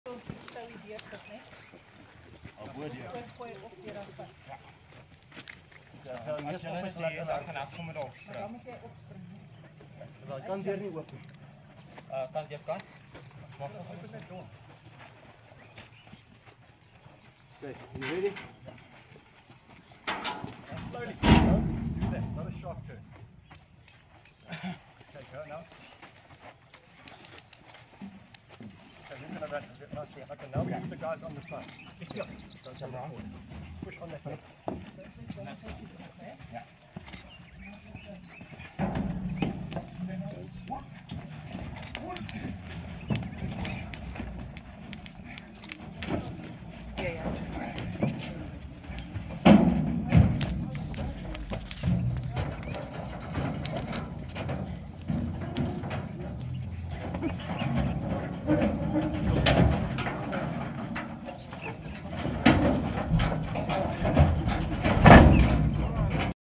BIG NEWS: Rhinos Lula and Khulula to be released into the wild
BIG NEWS‼️ Lula and Khulula to be released back into the wild. 🦏🦏🎉🎉 We are thrilled to announce that our two special ladies, Lula and Khulula, are leaving us today on their journey to be released into the wild. This video shows Khulula being guided into the trailer.